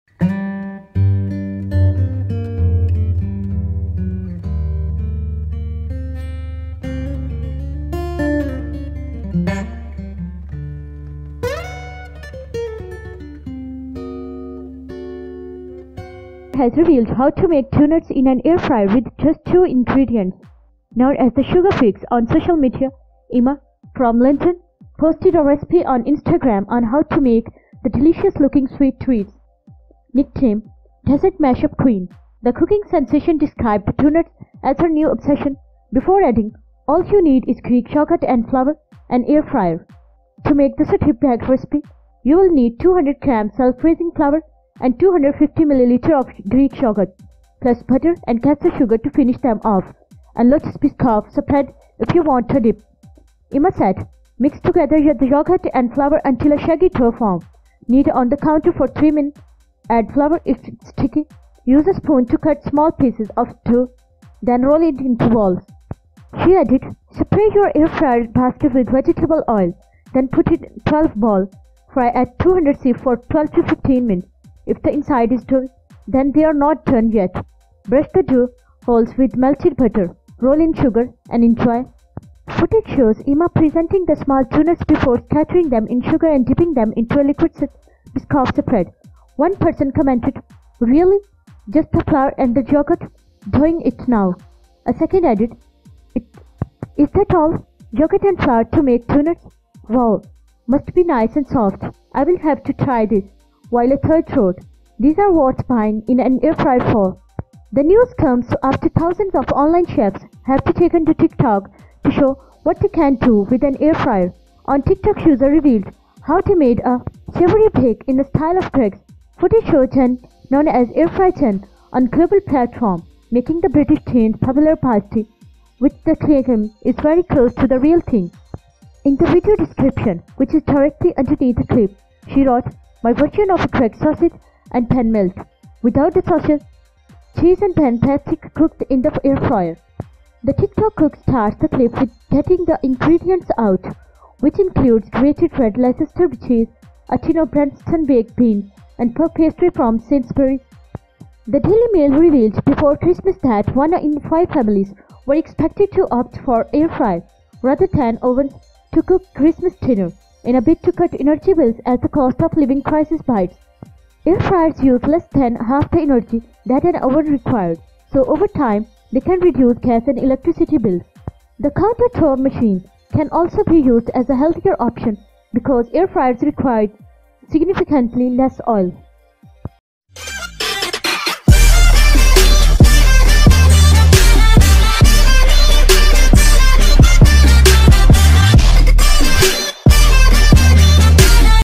A food blogger has revealed how to make doughnuts in an air fryer with just two ingredients
Known as The Suga Fix on social media, Emma, from London, posted a recipe on Instagram on how to make the delicious-looking sweet treats.

Nicknamed 'Dessert Mash-up Queen', the cooking sensation described the doughnuts as her 'new obsession' before adding 'all you need is Greek yoghurt & flour…& an air fryer!'

To make the stripped-back recipe you will need 200g self raising flour and 250ml of Greek Yoghurt - plus butter and caster sugar to finish them off, and Lotus Biscoff spread if you want a dip.

Emma said: 'Mix together the yoghurt and flour until a shaggy dough forms. Knead on the counter for 3 mins, add flour if it's sticky. Use a spoon to cut small pieces of dough, then roll into balls.

She added:  'Spray your air fryer basket with vegetable oil, then put in 12 balls. Fry at 200c for 12-15 mins (if the inside is doughy they are not done yet).

'Brush the dough holes with melted butter, roll in sugar & enjoy!'

Footage shows Emma presenting the small doughnuts before scattering them in sugar and dipping them into a liquidised Biscoff spread.

One person commented: 'Really? Just the flour and yoghurt.. doing it now.'

A second added: 'Is that all? Yogurt and flour to make donuts? Wow! Must be nice and soft! I'll have to try this.'

While a third wrote: 'These are worth buying an air fryer for!'

The news comes after thousands of online chefs have taken to TikTok to show what they can do with an air fryer.

One TikTok user revealed how they made a savoury bake in the style of Greggs.

Footage shows Jen, known as @airfry_jen on the global platform, making the British chain's popular pasty - which she claims is very close to the real thing.

In the video description, which is directly underneath the clip, she wrote:  'My version of a Greggs sausage & Bean melt... without the sausage... Cheese and Bean Pasty cooked in the air fryer.

The TikTok cook starts the clip with getting the ingredients out, which includes grated Red Leicester cheese, a tin of Branston baked beans and Puff Pastry from Sainsbury's.

The Daily Mail revealed before Christmas that one in five families were expected to opt for air fryers rather than ovens to cook Christmas dinner in a bid to cut energy bills as the cost-of-living crisis bites.